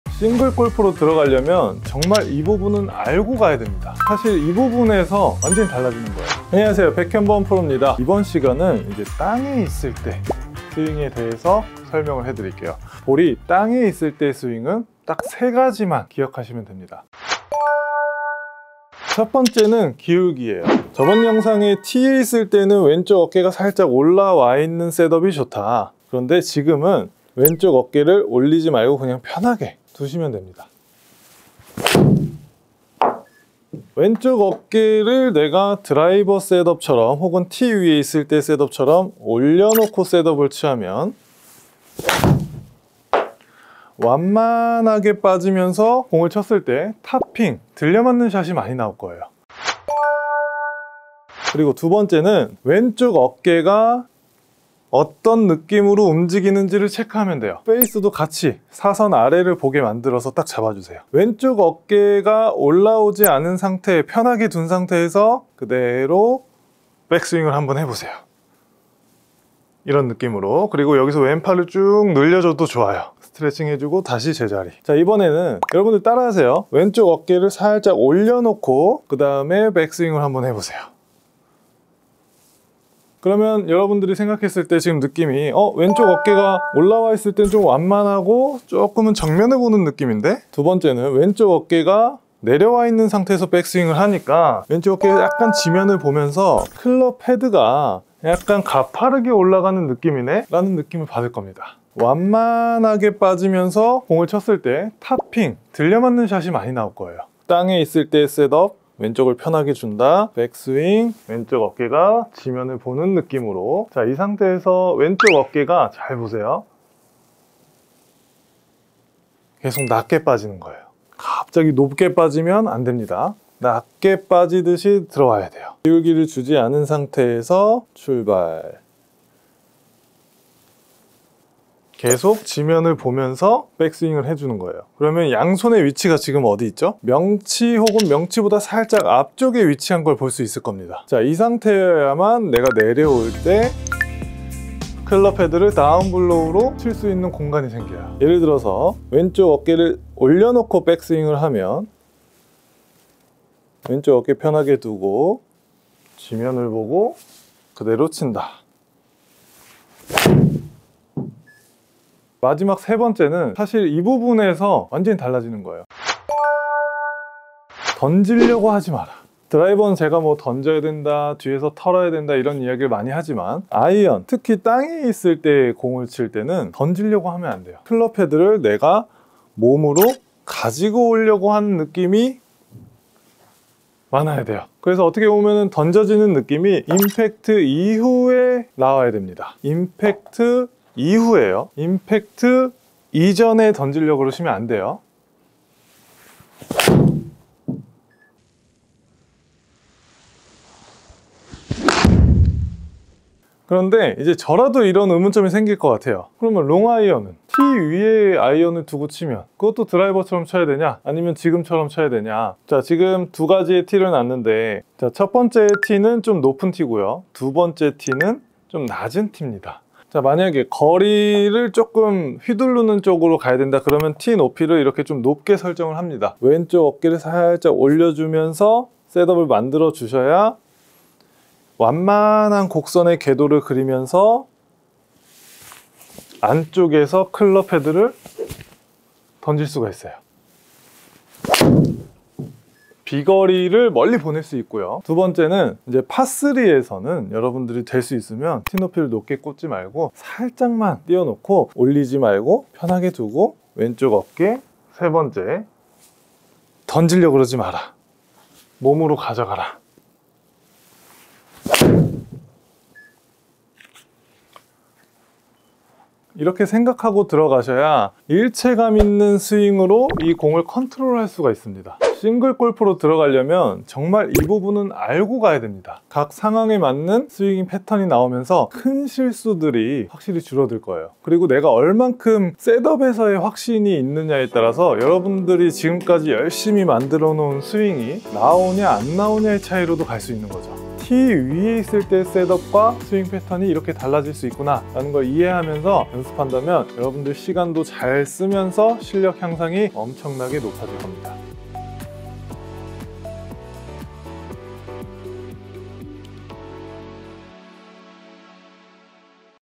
이렇게 하니 기가막하게 내려쳐집니다 : 셋업부터 스윙까지 한방에 정리!
공이 땅위에 있을때 셋업부터 스윙
이것 3가지는 꼭 지키며 연습해보세요!